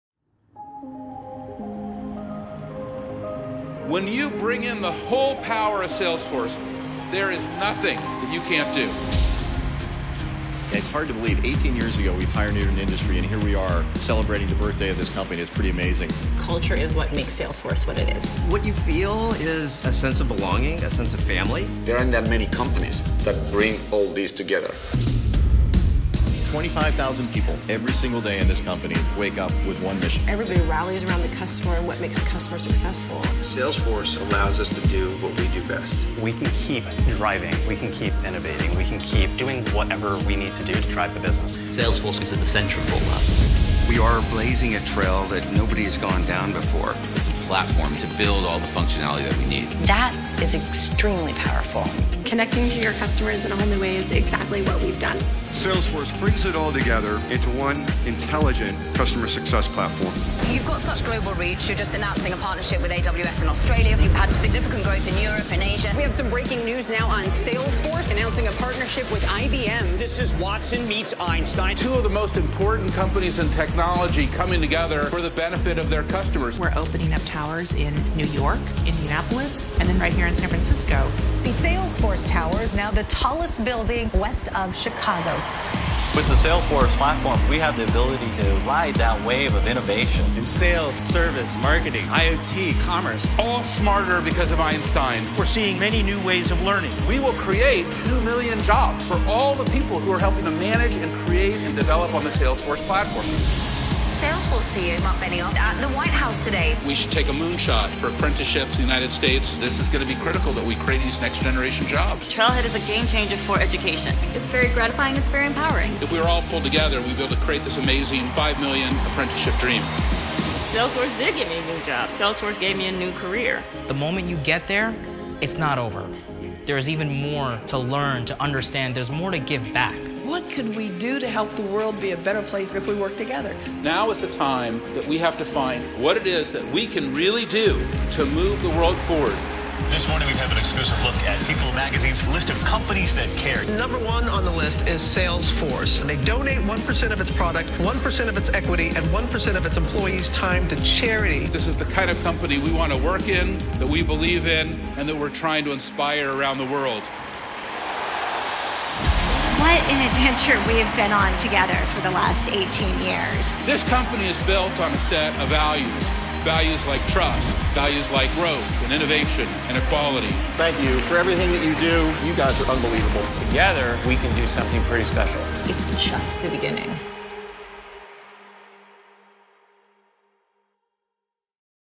Salesforce FY18Q1 Highlights
Thank you for an exciting Q1! Together, we are creating a better world. Salesforce was listed #1 in People Magazine’s list of “50 Companies That Care,” and we announced a historic partnership with IBM to lead the way in artificial intelligence.